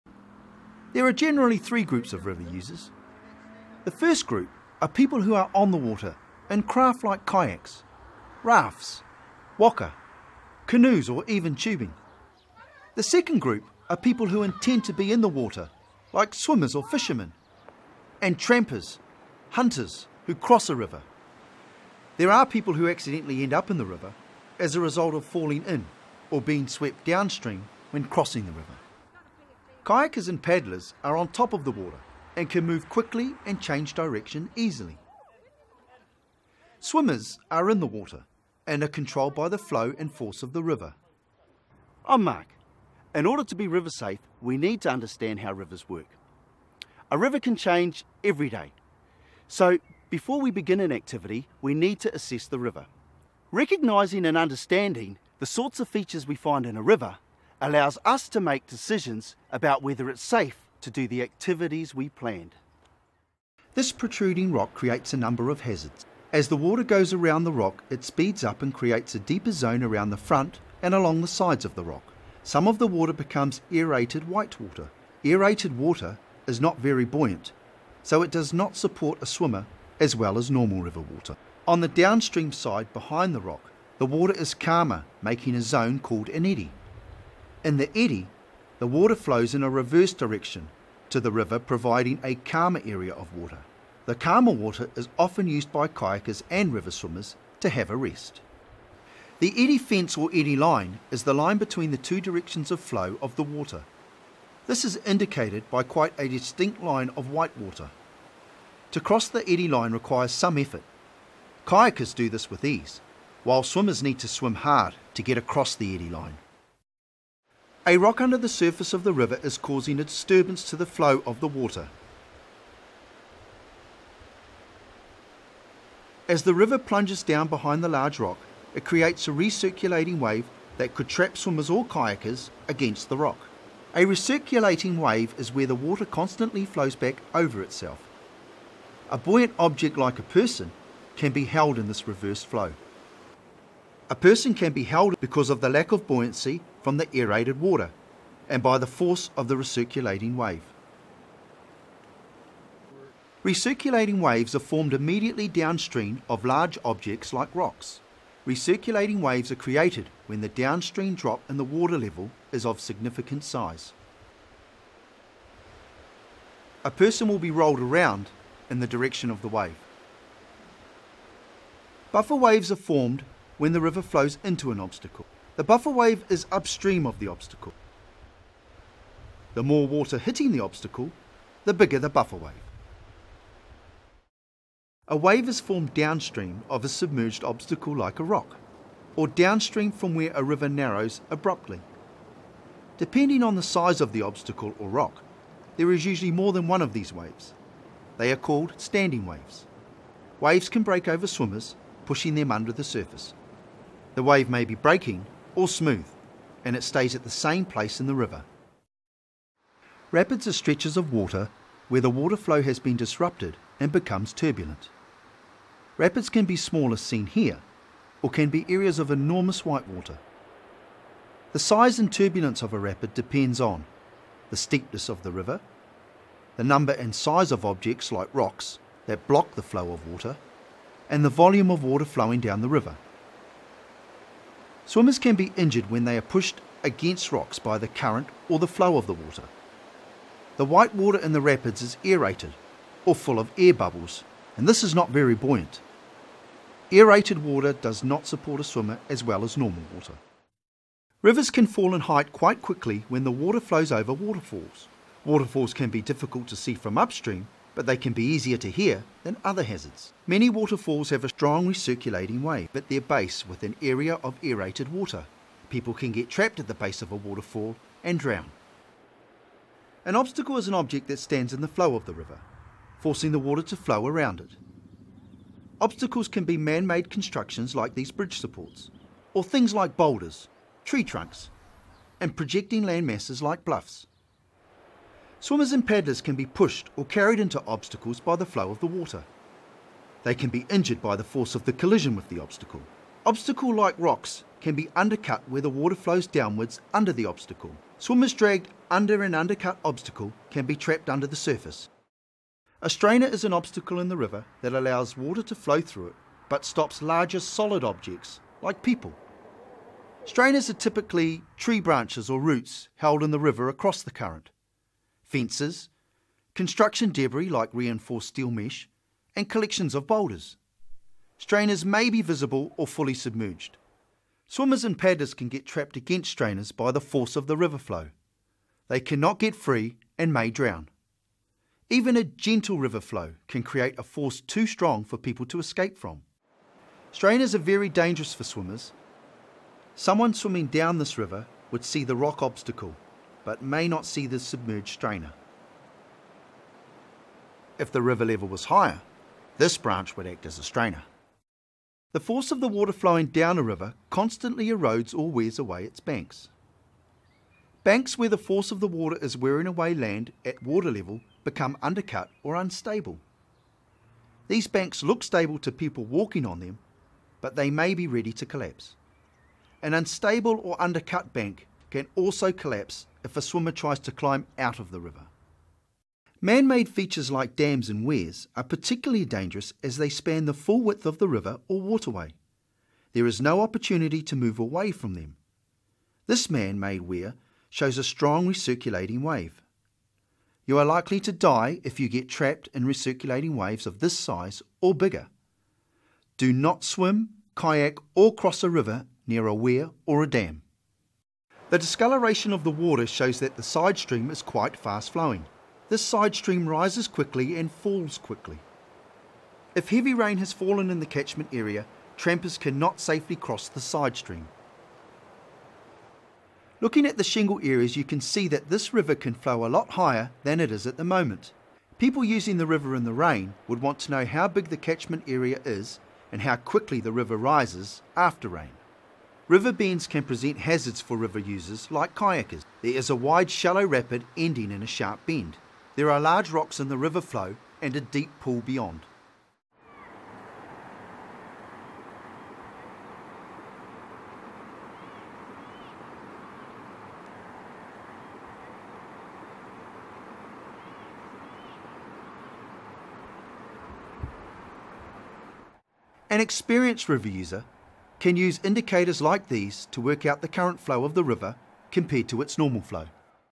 River Features 8 min
A short video that outlines important river features, how to spot them and what to avoid.